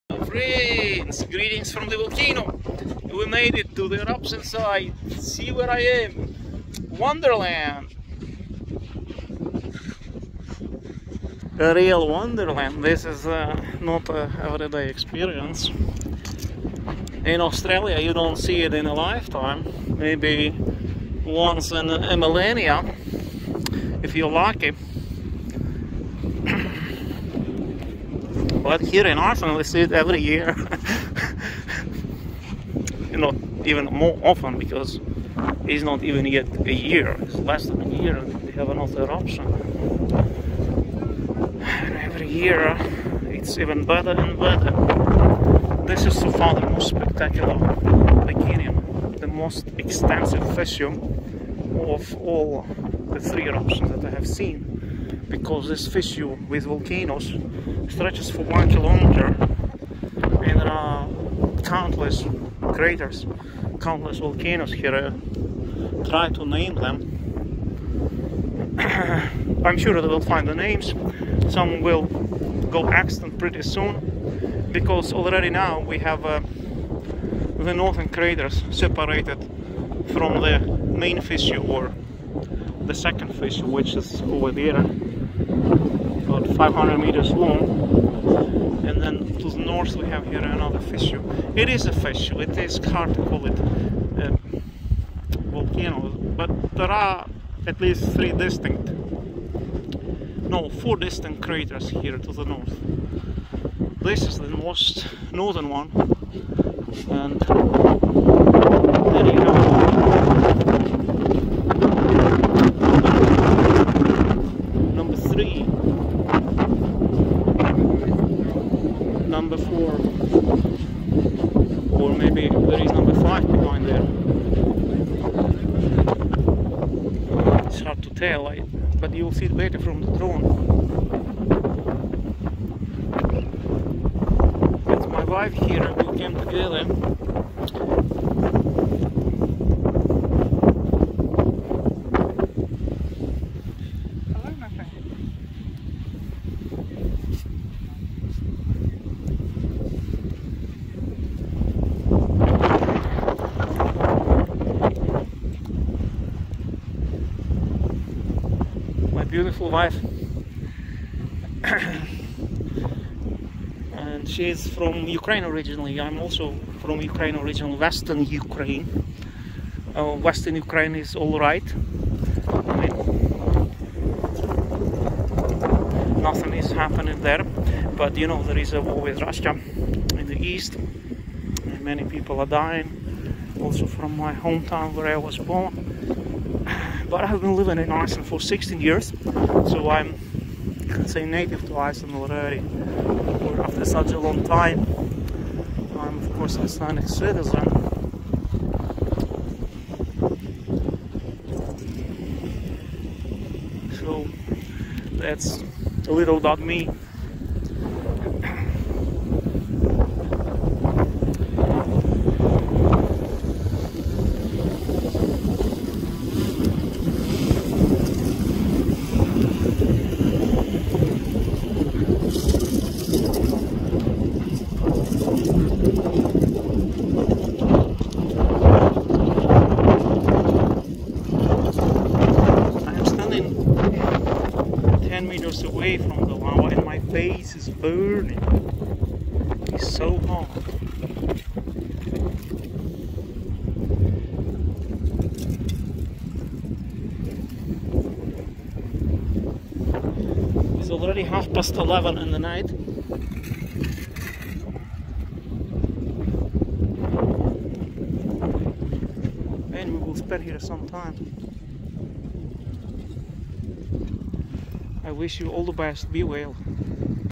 Hot lava flows running over moss terrain in all directions. See how close we came. 10.07.23
Volcano report from Litlihrutur eruption site. July 10, 2023. Iceland.